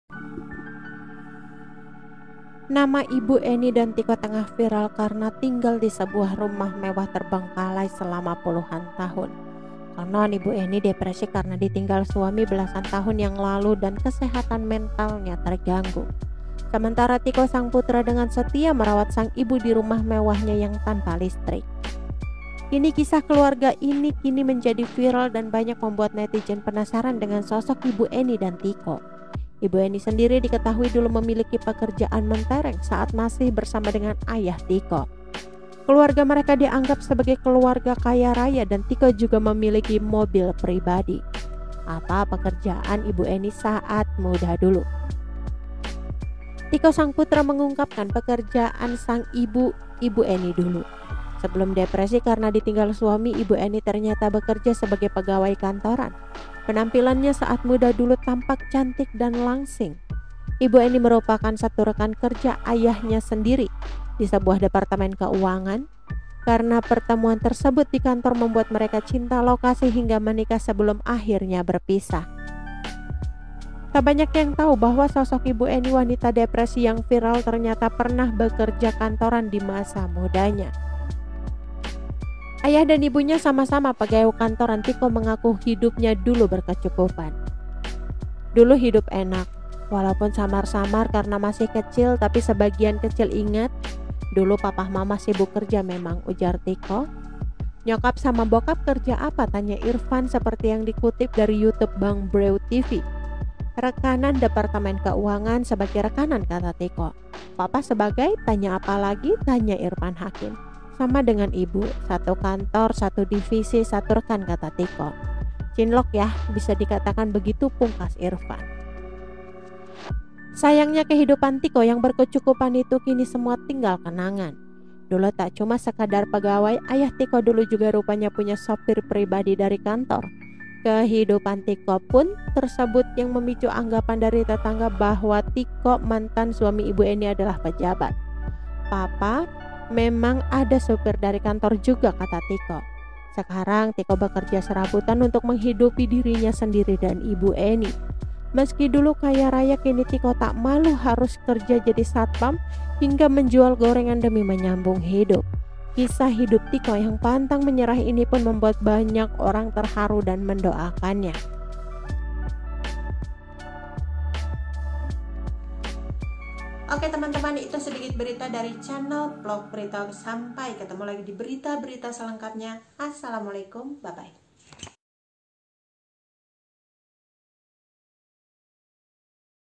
Momen Masa Muda Ibu Eny, Mama dari Tiko yang Viral, Dulu Pegawai Kantoran Cantik Rekanan Suami
Nama Ibu Eny dan Tiko tengah viral karena tinggal di sebuah rumah mewah terbengkalai selama puluhan tahun.

Konon, Ibu Eny depresi karena ditinggal suami belasan tahun yang lalu dan kesehatan mentalnya terganggu.

Sementara Tiko, sang putra, dengan setia merawat sang Ibu di rumah mewahnya yang tanpa listrik.

Artikel ini telah tayang di TribunNewsmaker